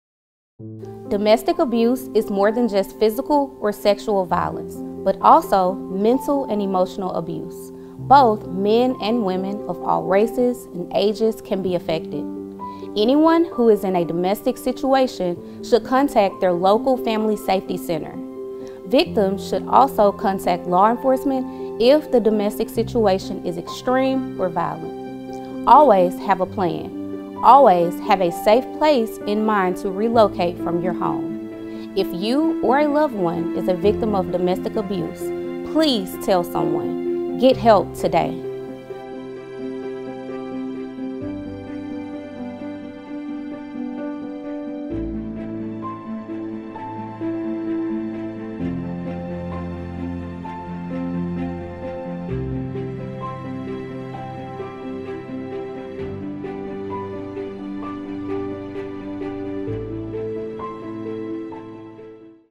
Domestic Abuse PSA from the Shelby County Sheriff's Office
If you or someone you know is experiencing domestic abuse, contact the Family Safety Center of Memphis and Shelby County at or the Shelby County Sheriff’s Office Domestic Violence Unit at for help. Remember, you’re not alone.